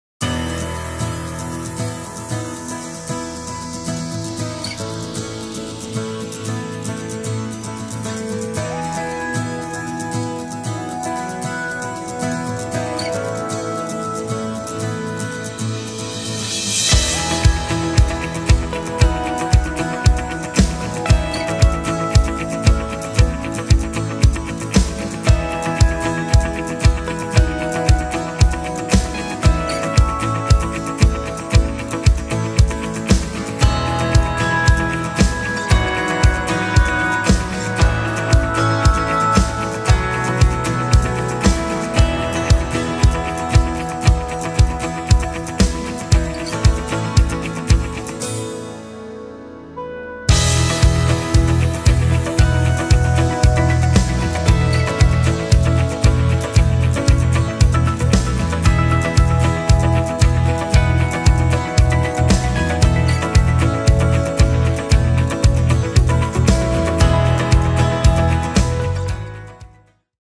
Moweo 46.5 Li | Battery lawnmower | AL-KO Gardentech UK
The AL-KO Moweo 46.5 Li impresses with its low weight, which prevents mowing fatigue quickly. Thanks to innovative components such as the lithium-ion battery with charge level indicator, the AL-KO Moweo 46.5 Li is a reliable partner for your garden and is supplied with two batteries and 1 charger.

Moweo 46.5 Li | Battery lawnmower